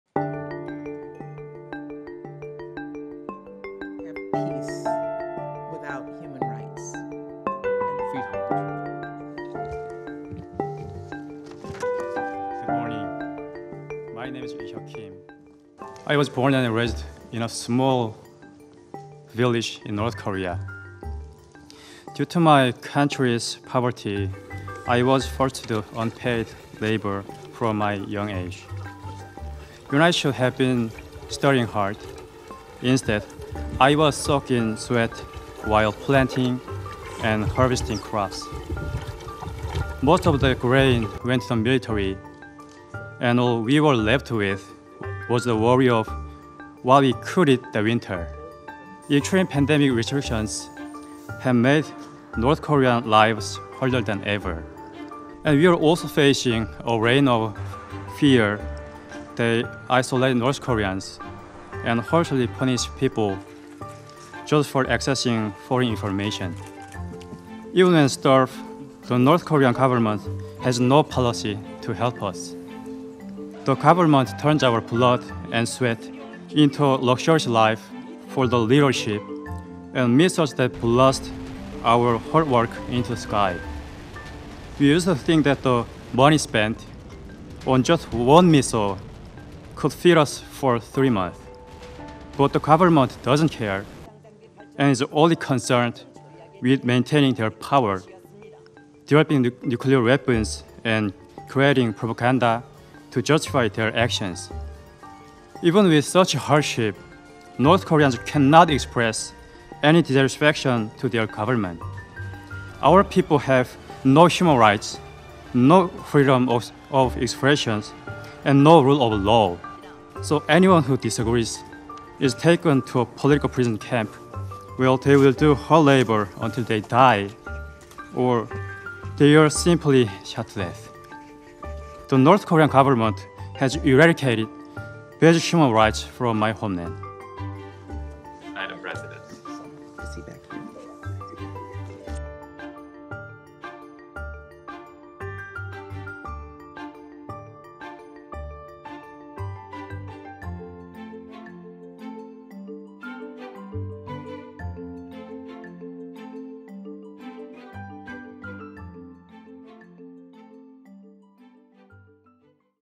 North Korean defector describes brutality of Kim regime
North Korean defector Ilhyeok Kim addressed the United Nations Security Council to tell his story of forced labour and routine hunger in the winter. North Korea had voiced its opposition on August 15, 2023, to the meeting about its human rights record, accusing the United States of advancing its own agenda by leading the effort.